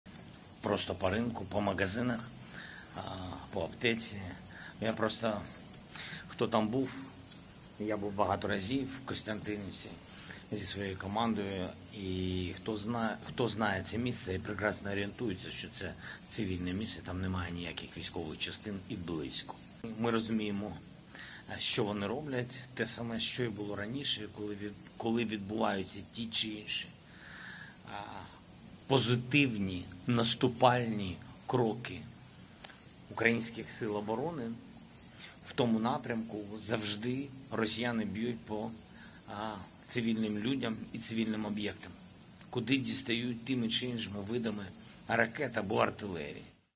Zelensky condemns Russian attack in eastern Ukraine
Ukrainian President Volodymyr Zelenskyy has condemned a Russian missile strike in eastern Ukraine which has killed at least 17 people.

Speaking at a press conference in Kyiv, Zelenskyy said there was no military base ‘anywhere near’.